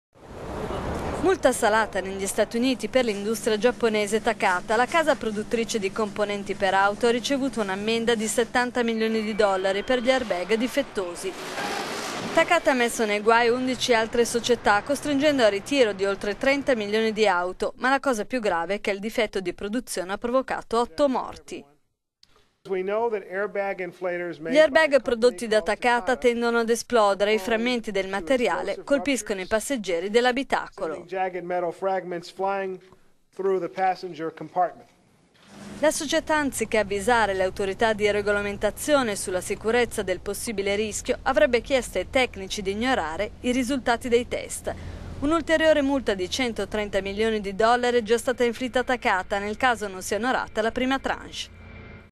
Usa: multata Takata per gli airbag difettosi
Multa salata negli Stati Uniti per l'industria giapponese Takata.
La casa produttrice di componenti per auto ha ricevuto un' ammenda di 70 milioni di dollari per gli airbag difettosi.

La Takata ha messo nei guai 11 altre società, costringendo al ritiro di oltre 30 milioni di auto. Ma la cosa più grave è che il difetto di produzione ha provocato 8 morti.

"Gli airbag prodotti da Takata tendono ad esplodere - ha spiegato Anthony Renard Foxx, segretario Usa dei trasporti - e i frammenti del mate…